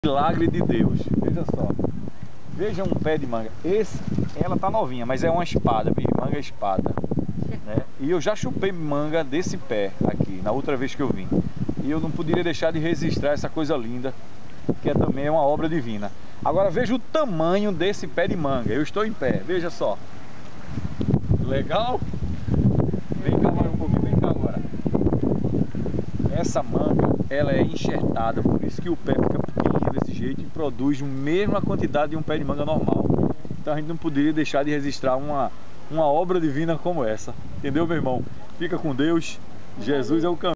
CAL E A MANGUEIRA ENXERTADA
MANGUEIRA ENXERTADA PRODUZ FRUTO RAPIDINHO E DE BOA QUALIDADE.